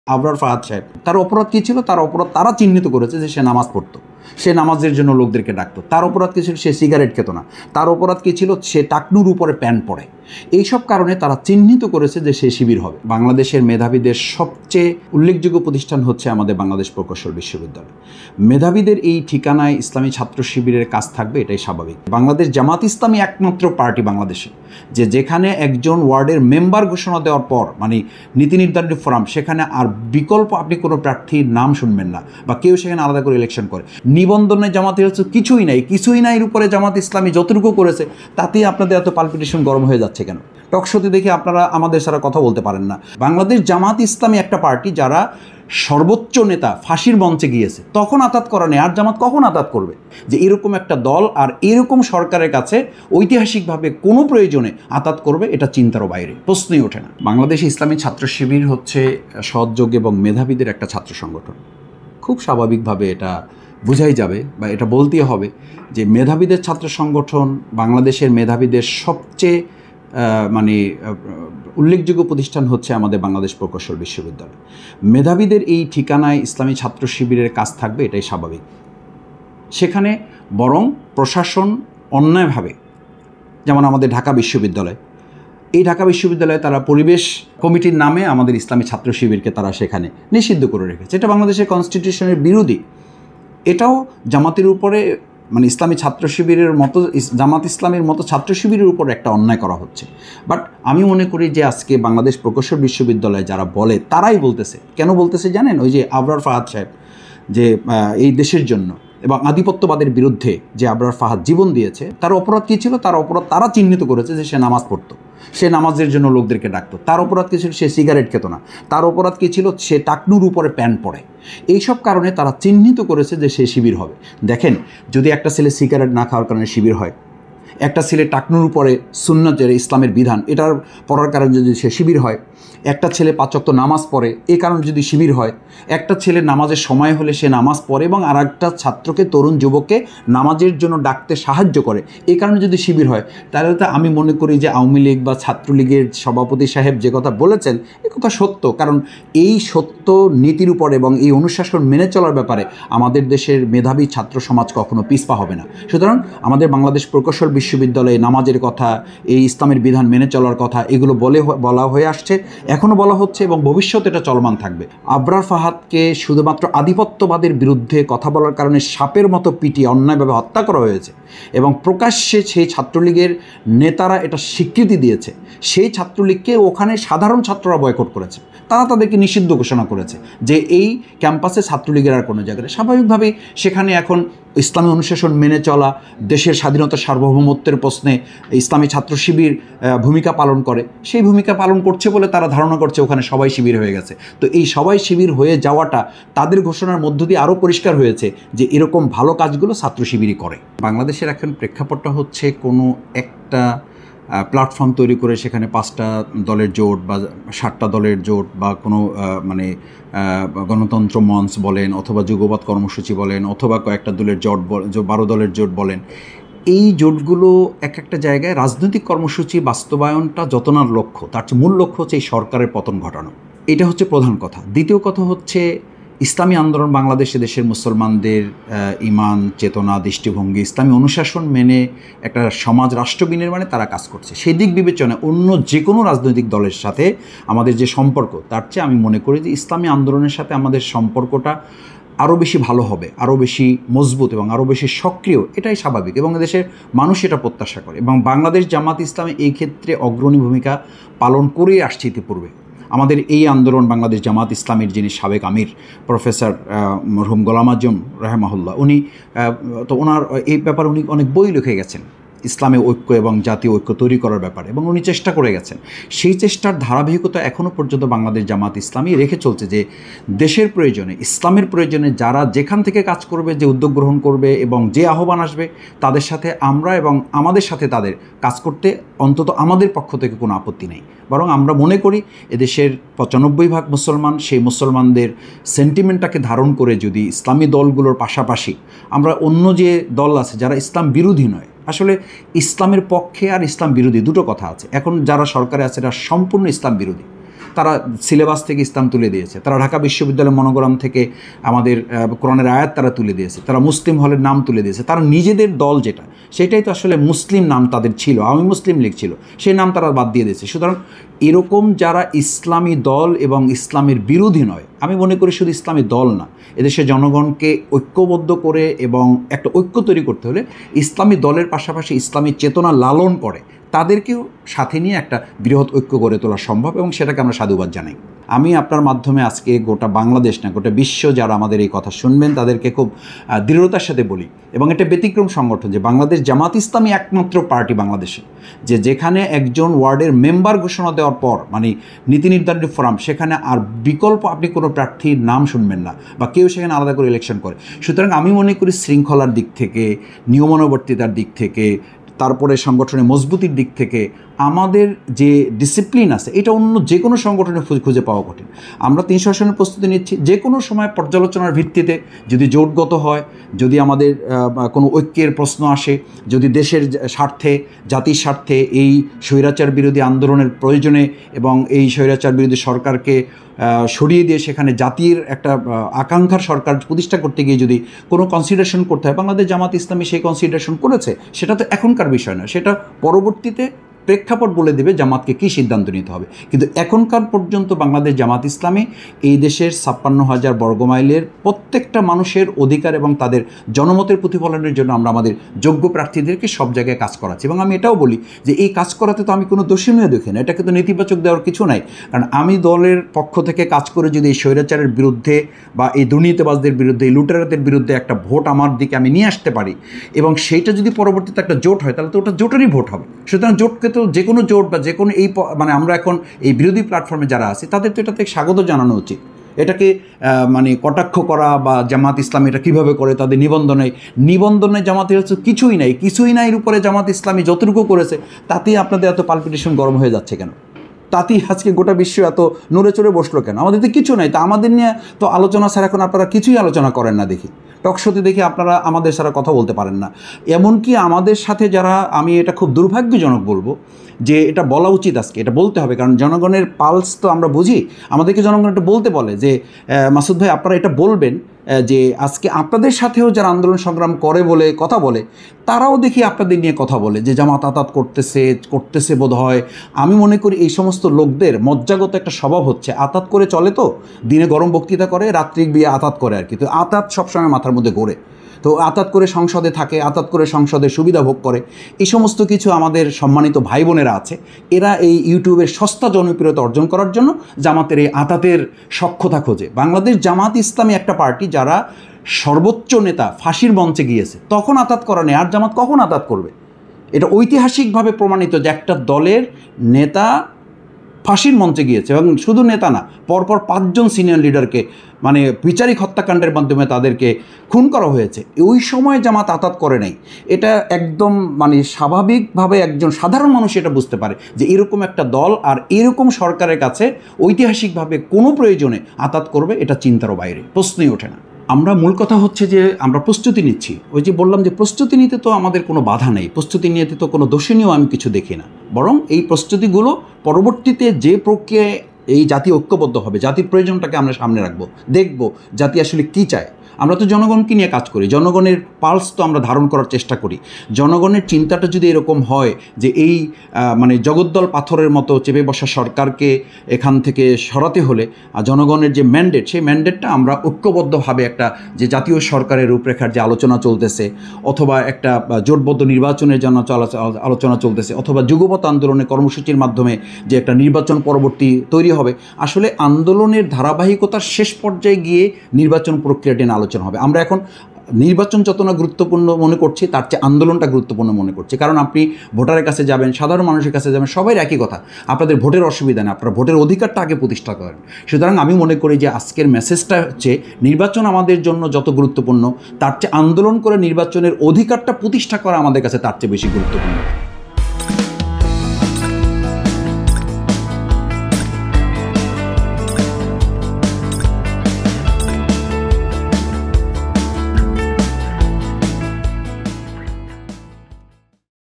বুয়েটে ছাত্র শিবিরের রাজনীতি নিয়ে যা বললেন ড. শফিকুল ইসলাম মাসুদ | Dr Shofiqul Islam | Jaijaidin
বুয়েটে ছাত্র শিবিরের রাজনীতি নিয়ে যা বললেন ড. শফিকুল ইসলাম মাসুদ